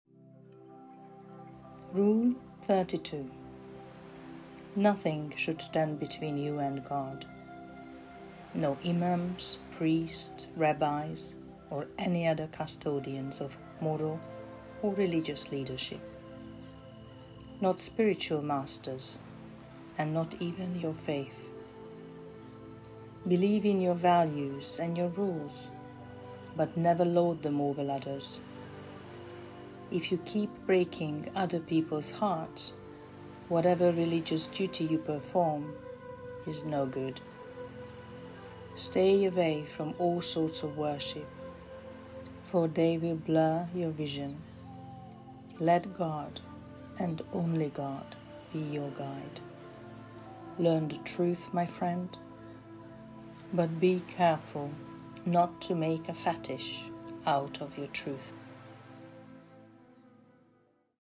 40 Rules of Love - Rule 32
Nothing should stand between you and God. No imams, priests, rabbis, or any other custodians of moral or religious leadership. Not spiritual masters and not even your faith. Believe in your values and your rules, but never lord them over others. If you keep breaking other people’s hearts, whatever religious duty you perform is no good. Stay away from all sorts of worship, for they will blur your vision. Let God and only God be your guide. Learn the Truth, my friend, but be careful not to make a fetish out of your truths.